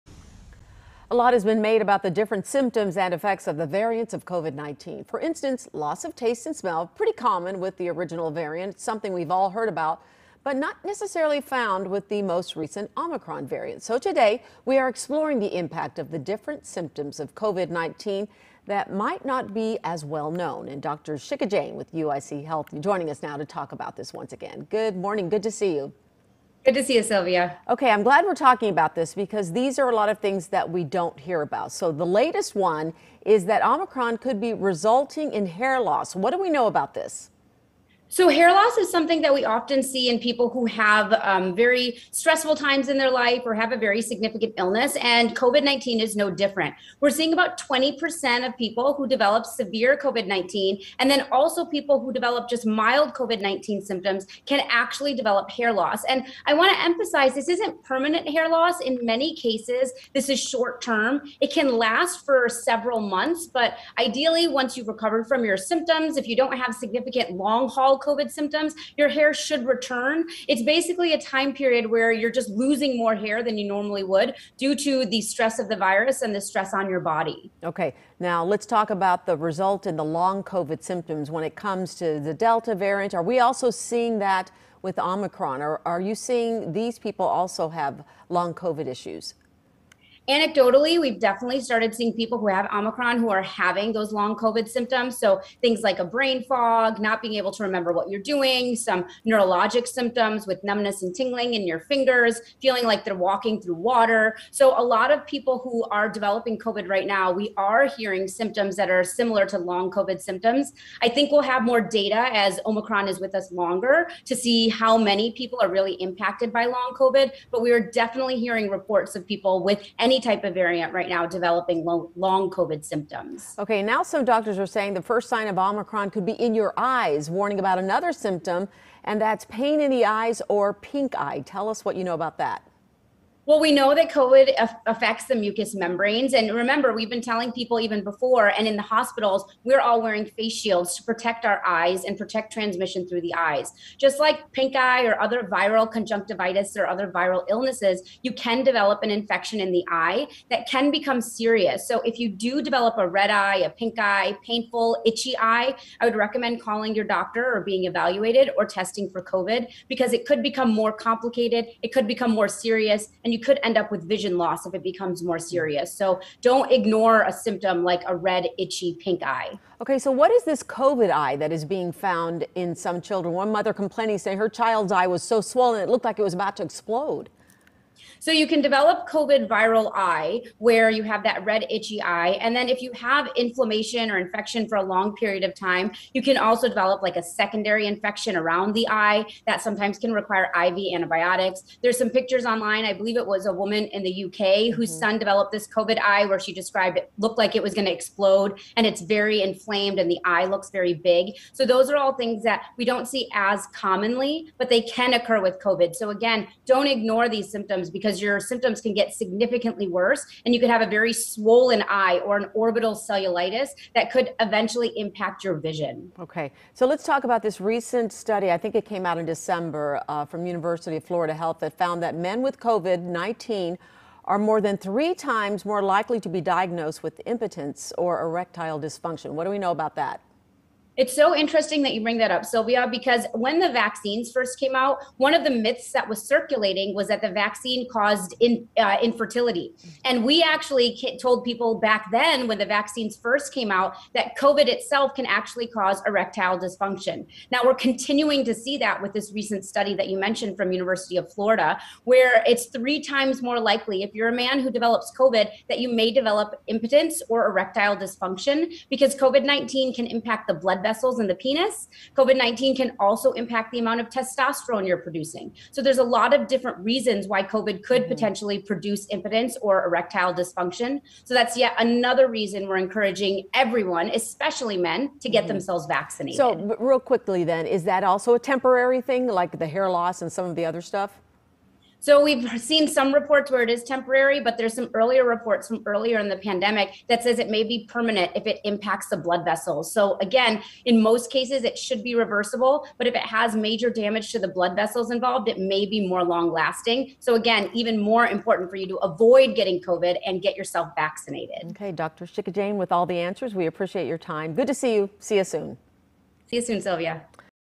What are some of the different symptoms of the Omicron variant?
Dr. Shikha Jain from UI Health walks us through some of the distinctive symptoms of the Omicron variant and how they can be observed.